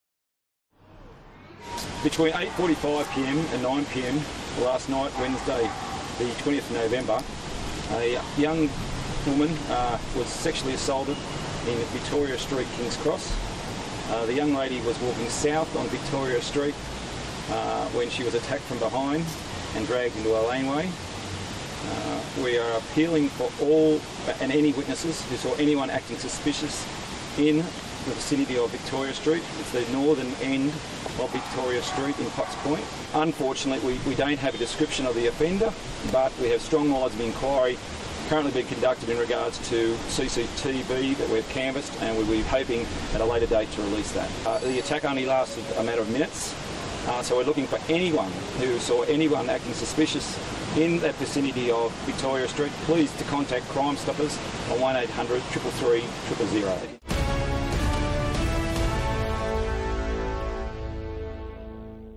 Police appeal in tourist assault case
NSW Police appeal for help form people who might have been in the Potts Point area around the time a Belgian tourist was sexually assaulted early on Wednesday night.